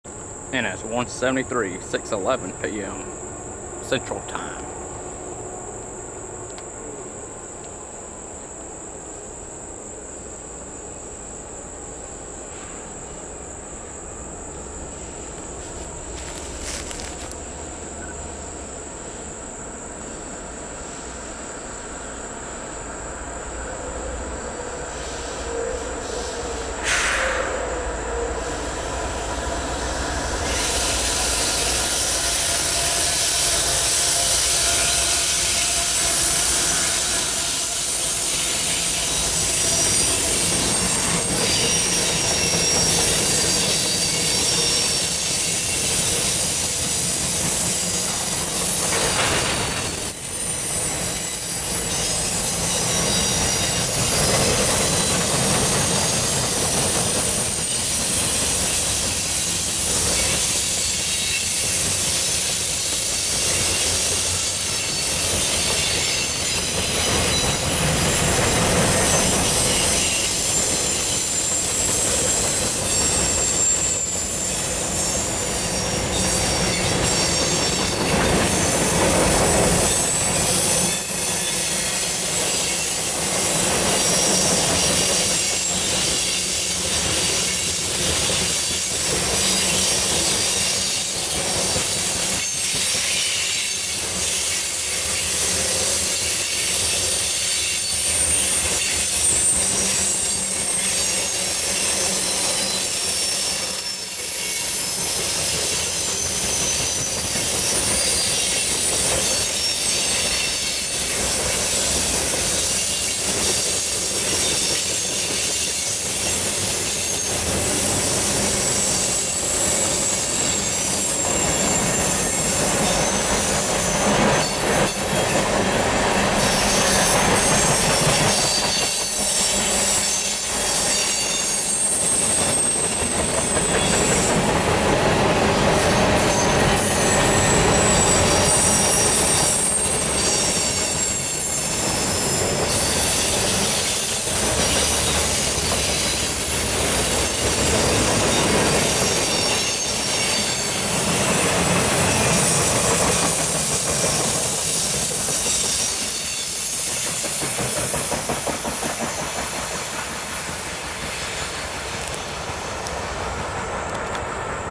HD: NS 8036 leads NS Train 173 in Fruithurst, AL
Milepost 706.2
Fruithurst
9/5/2016
6:11p.m. C.T.
NS: 173
NS: 8036(ES44AC)
NS: 8102(ES44AC) Pennsylvania Heritage Unit
NS: 8840(D9-40C)
Westbound
Mixed Freight